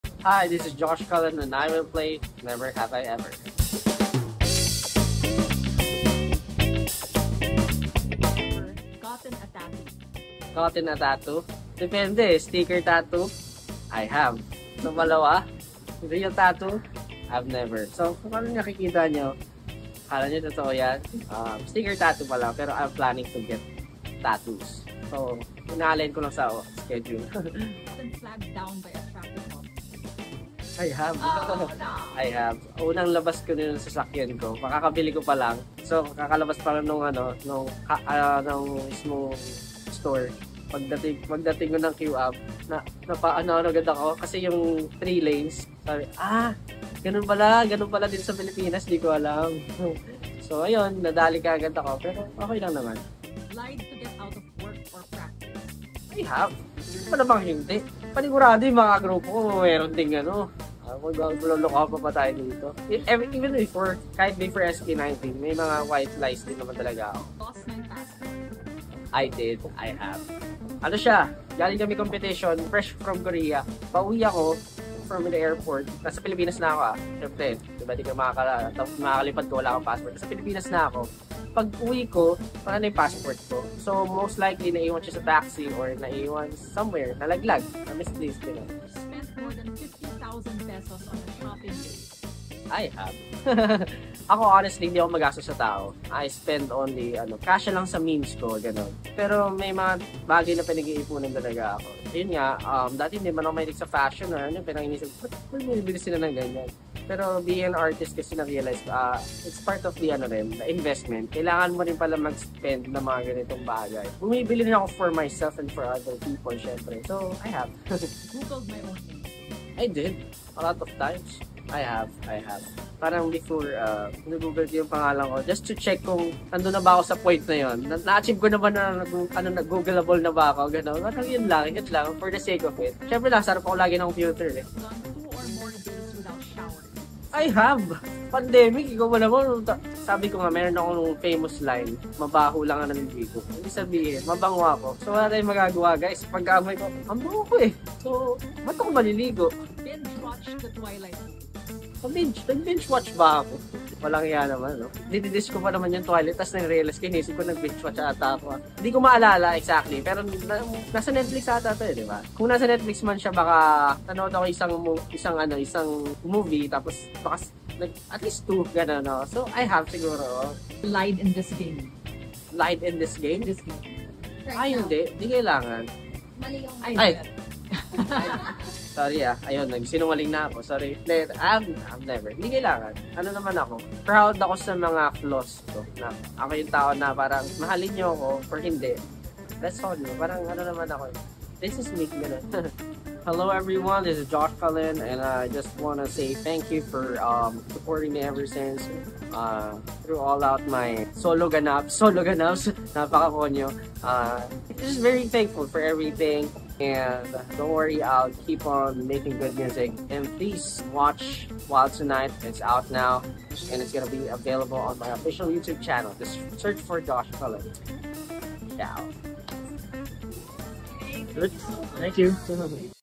Josh Cullen plays 'Never Have I Ever'
Singer-songwriter and SB19 member Josh Cullen gives us a peek into the little choices he makes in everyday life as he plays the classic game 'Never Have I Ever'.